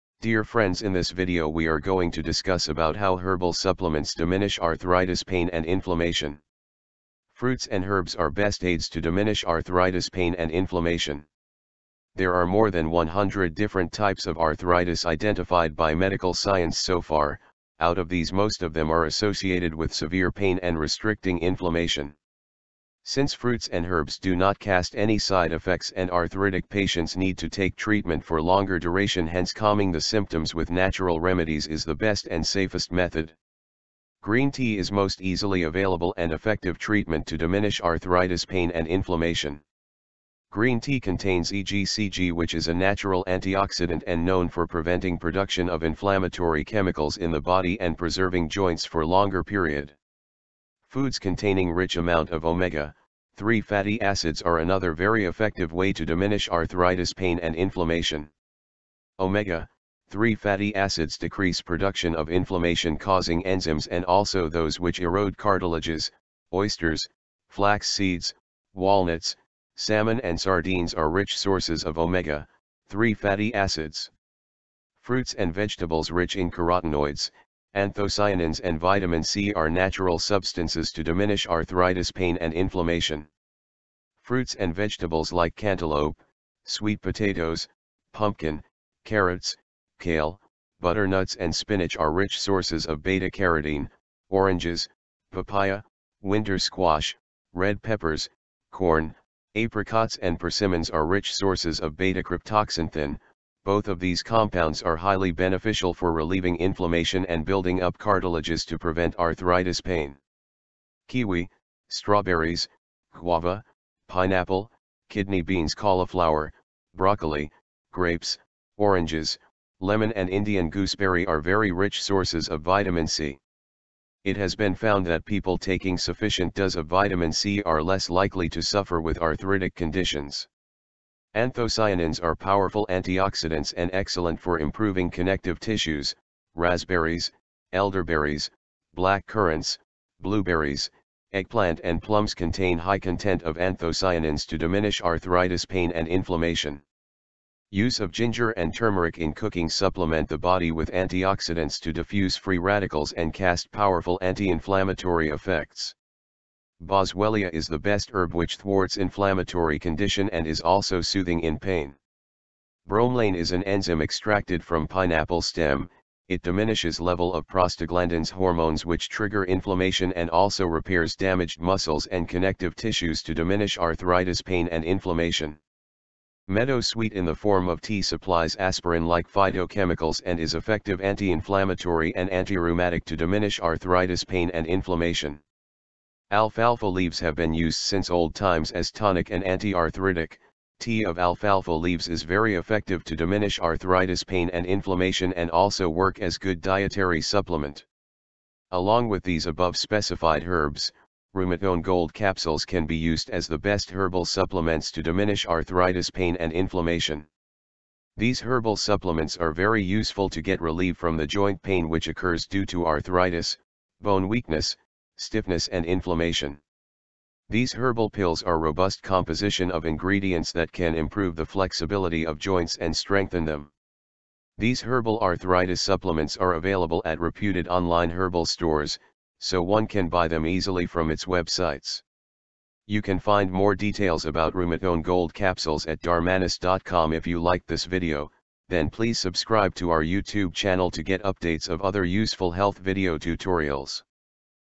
How Herbal Supplements Can Diminish Arthritis Pain And Inflammation?
Dear friends in this video we are going to discuss about how herbal supplements diminish arthritis pain and inflammation. You can find more details about Rumatone Gold capsules